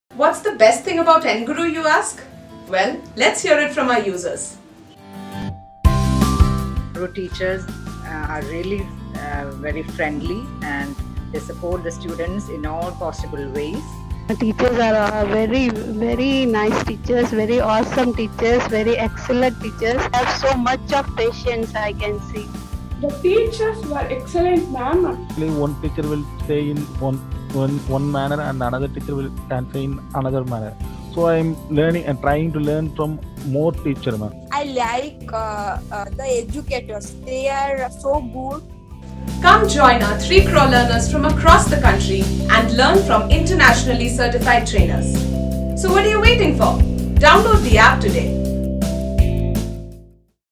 enguru User Reviews : What's Best About enguru
Let's meet some of enguru's awesome users from the 3 crore learners across the country and learn why the best thing that they like about enguru

Join our learners and next time we might feature you.

Happy Learning!